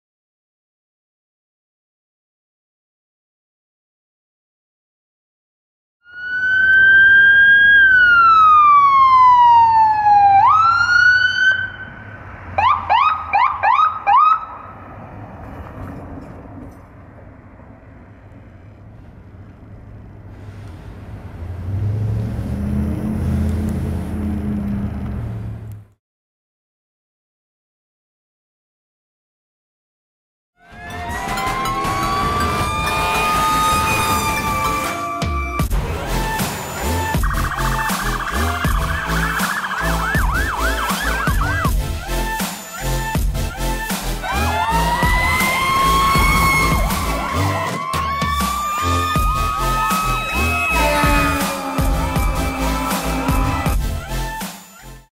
Volunteer Medical Service Corps Medic 345-14 Responding 1/22/22
Volunteer Medical Service Corps Medic 345-14 Responding to a medical in Lansdale Borough, Montgomery County PA.